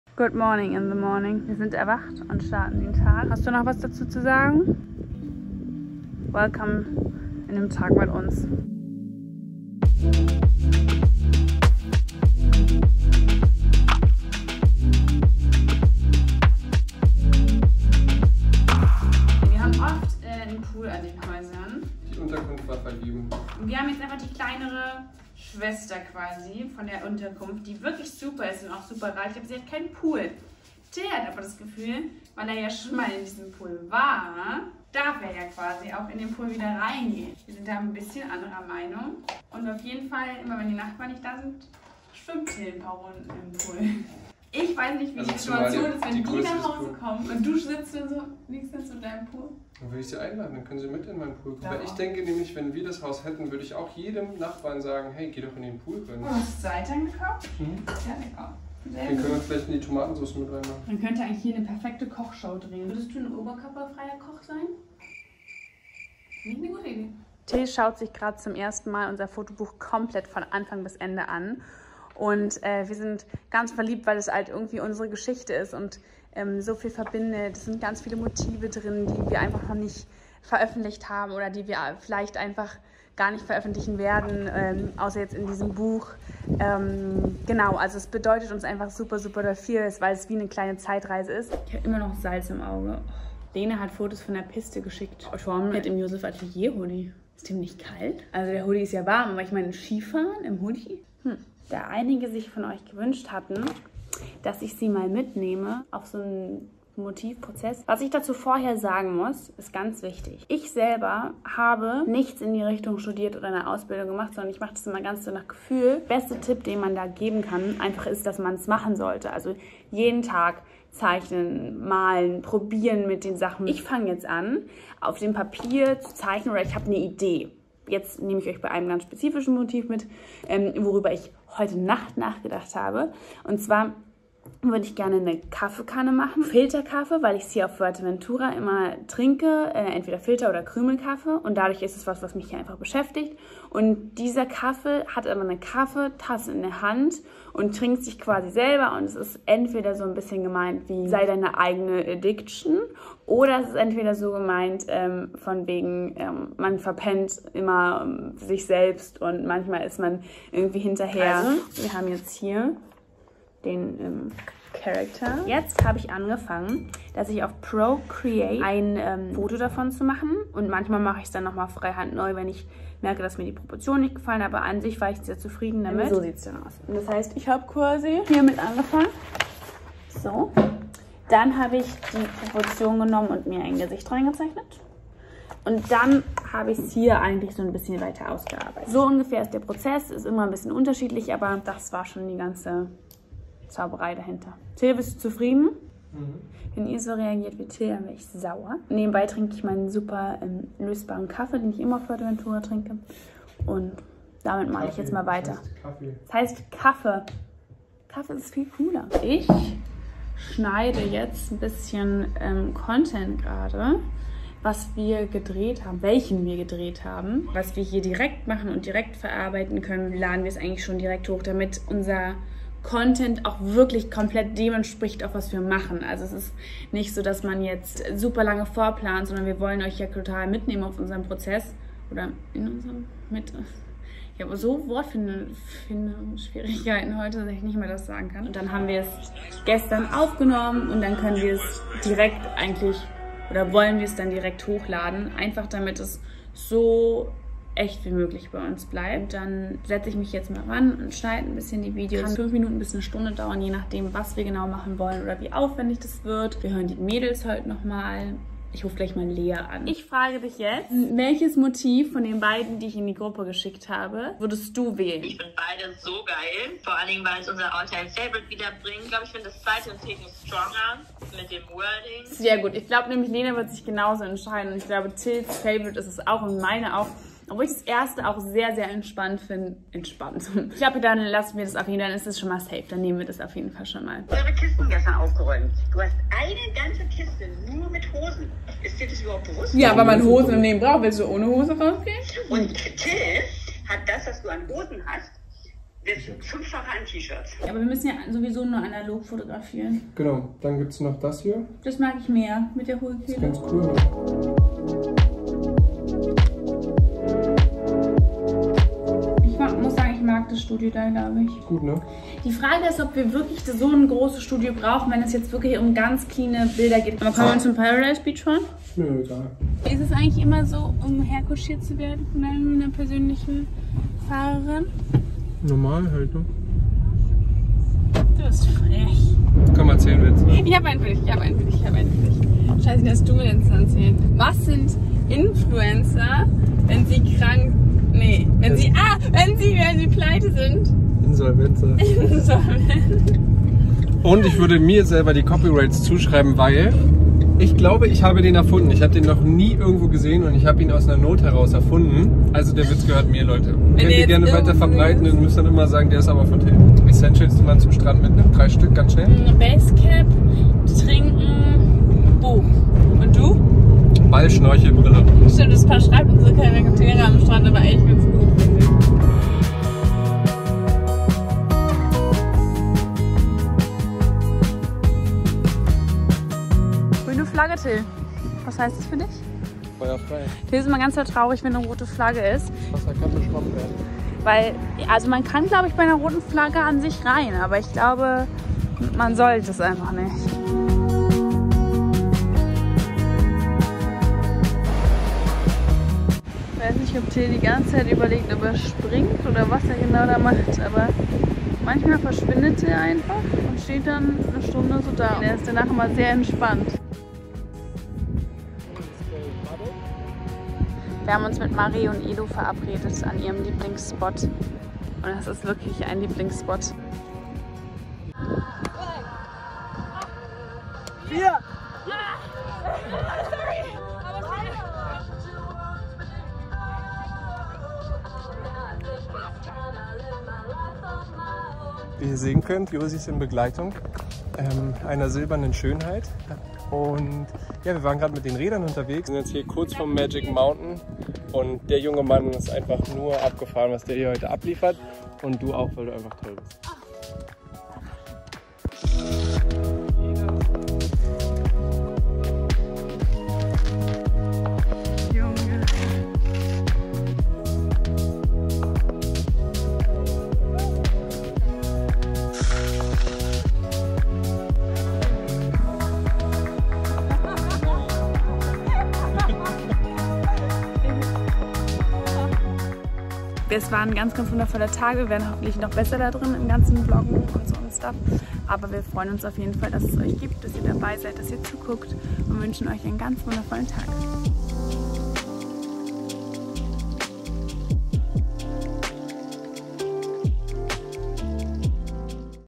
Vlog#003 - WORK - BEACH - BALANCE auf Fuerteventura
Hey Ihr Lieben, wir sind es wieder. Ihr hattet Euch gewünscht, dass wir Euch in Alltag mitnehmen. Also haben wir es versucht, Euch bestmöglich mitzunehmen. Für uns ist das noch alles sehr neu und wir freuen uns, immer mehr gemeinsam mit Euch zu wachsen und besser zu werden. Wir senden Euch ganz viel Liebe und hoffen Euch ein kleines Lächeln auf die Lippen zaubern zu können.

Wir freuen uns über Liebe und Support in den Kommentaren, Abos und Likes. Wir danken Euch für all Eure Liebe und Euren Support.

Joseph Atelier